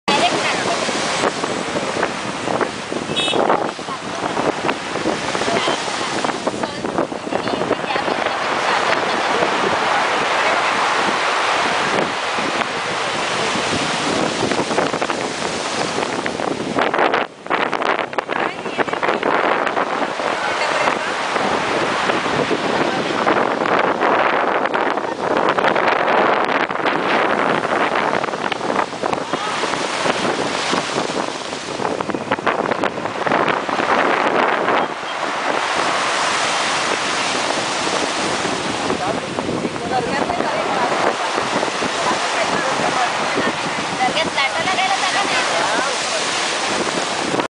Mumbai Hightide 24 07 09 9
videos of HighTide on 24-07-09 at Haji Ali